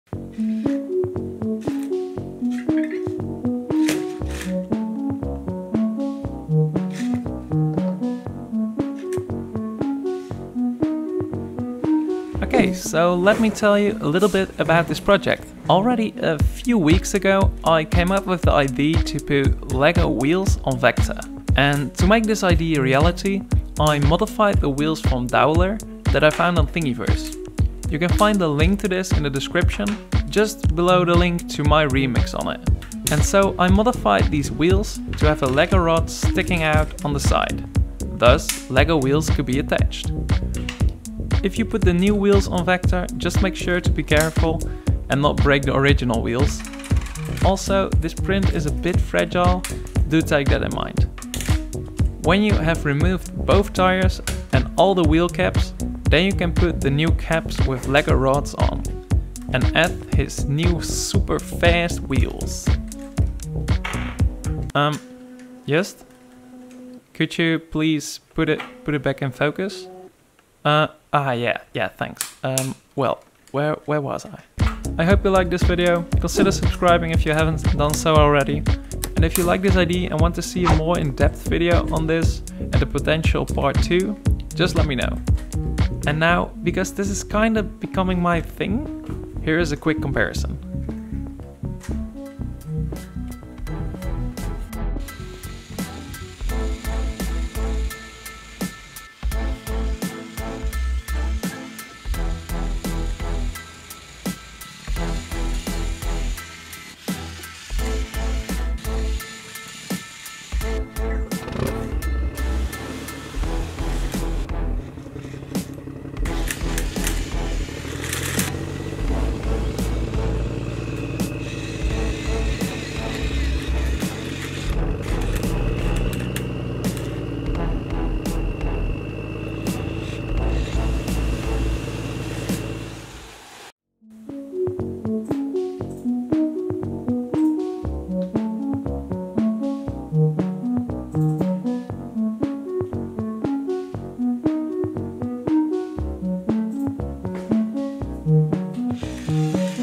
Awesome Vector Robot Lego Wheel Mod / Upgrade 3D printable
In this video I explain how to customize the Anki / DDL Vector robot to have Lego wheels instead of his normal tracks. I printed these on my Ender 3 in PLA but other filaments will probably also work fine. If tested the prints out please let me know how I can improve and hopefully I will see you then in next week’s video!